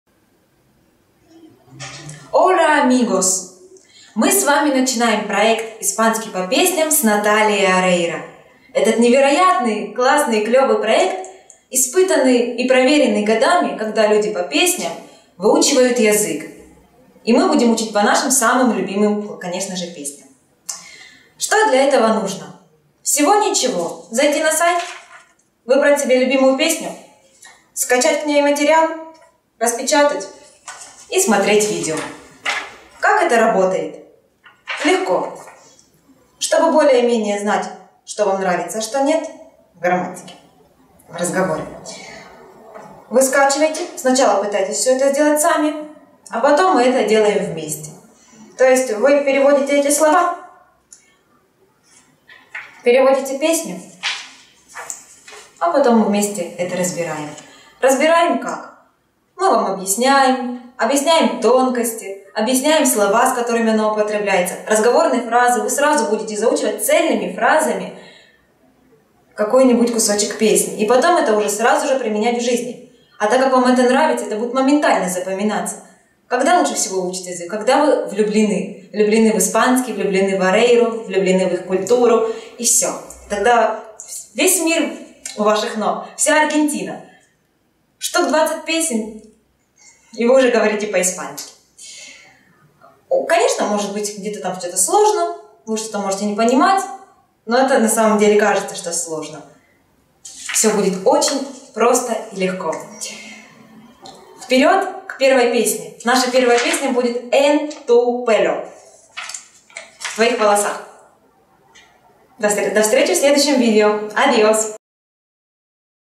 Introducción - Введение в изучение испанского языка
Что представляет собой изучение испанского по песням с Наталией Орейро?
И как это работает?